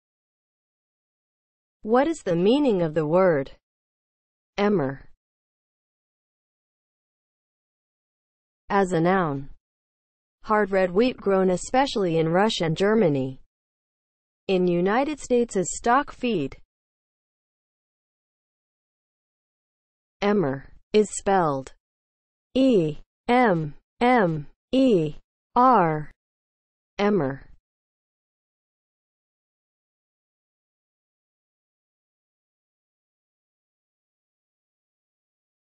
What is the meaning of the word EMMER?
Definition and spelling of the word EMMER.

(00:00) Intro
(00:07) As a noun
(00:18) Spelling

As a noun (00:07):
• hard red wheat grown especially in Russia and Germany; in United States as stock feed